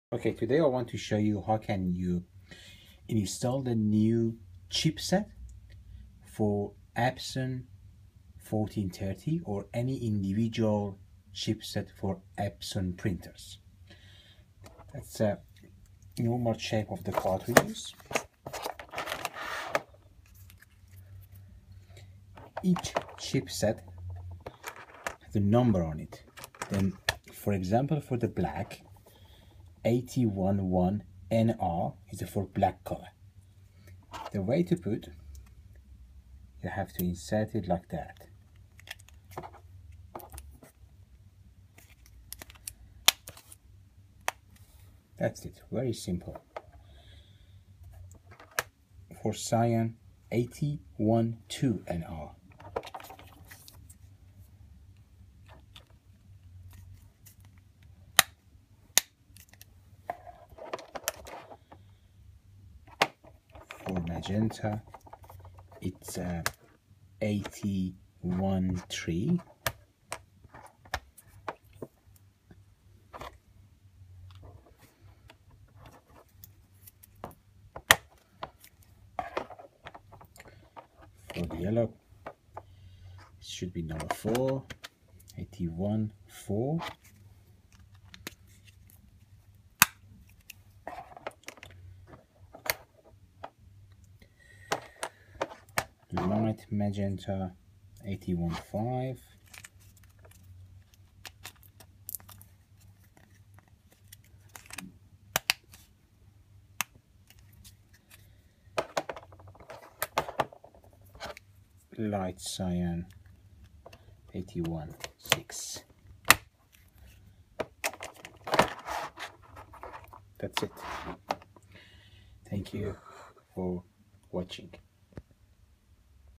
Change the new updated chip-set for ciss 1430 1410
Change the new updated  chip-set for ciss, support Epson printers: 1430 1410 T50